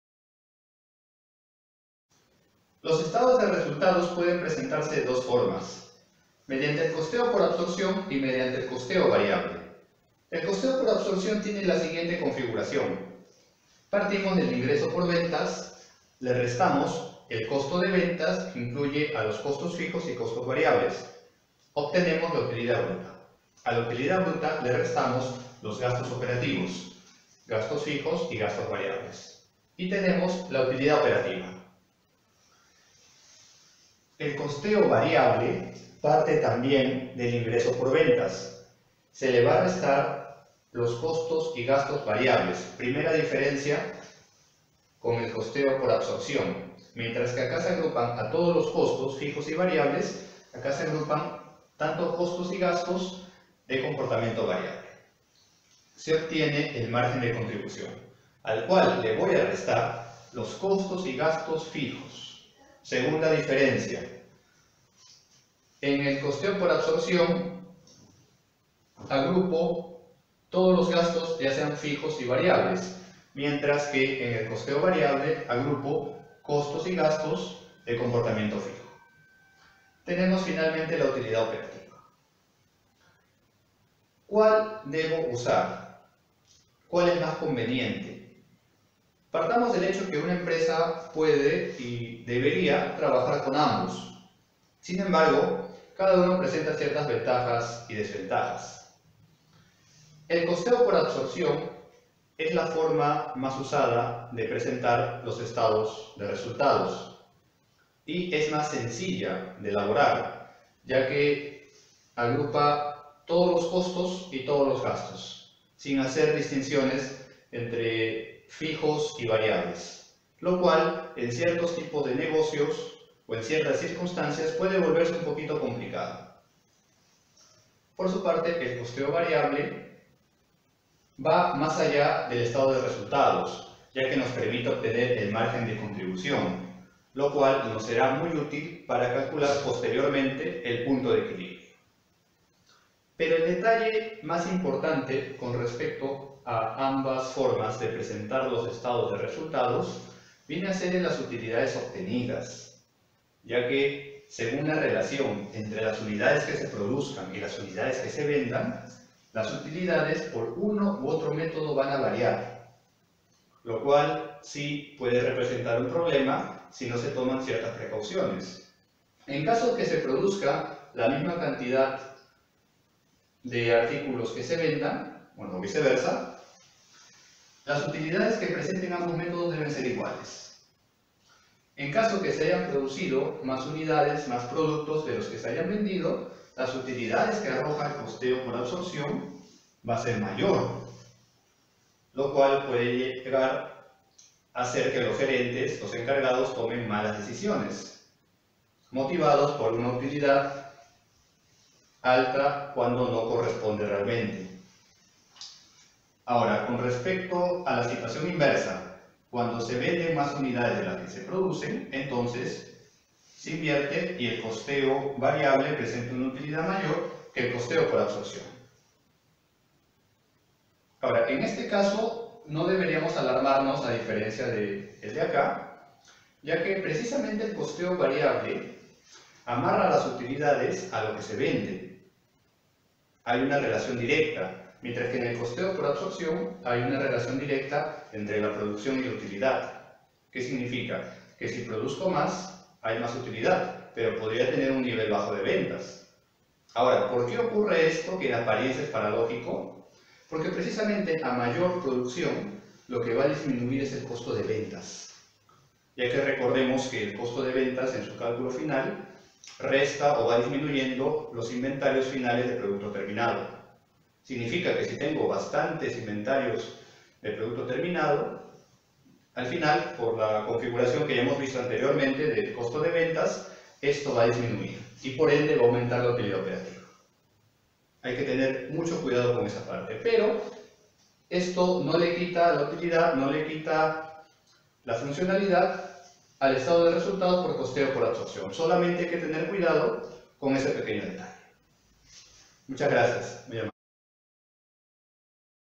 COSTEO ABSORCIÓN Y COSTEO VARIABLE
Lo esencial del Costeo por Absorción (Normal) y el Costeo Variable (Directo)

Expositor: Ing. Jorge Rosario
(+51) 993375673

Ver también:
Tutorial teoría de Costos - 1